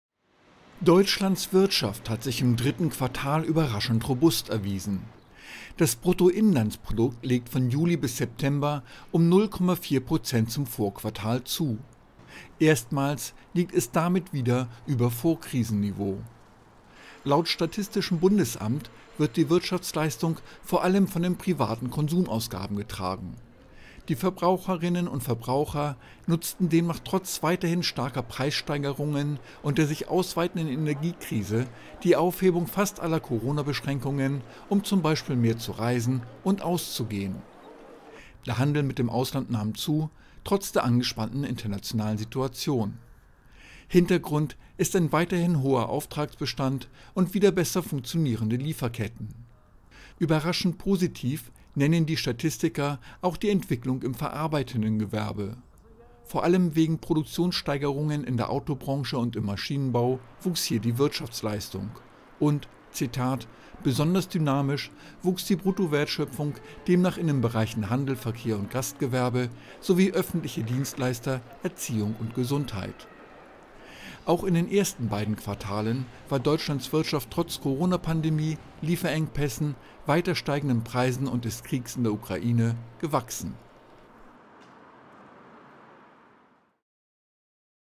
Deutschlands Wirtschaft trotz Krisen weiter gewachsen | AFP
Deutschlands Wirtschaft hat sich im dritten Quartal überraschend robust erwiesen: Das Bruttoinlandsprodukt (BIP) legte von Juli bis September um 0,4 Prozent zum Vorquartal zu und damit etwas mehr als in der ersten Schätzung berechnet.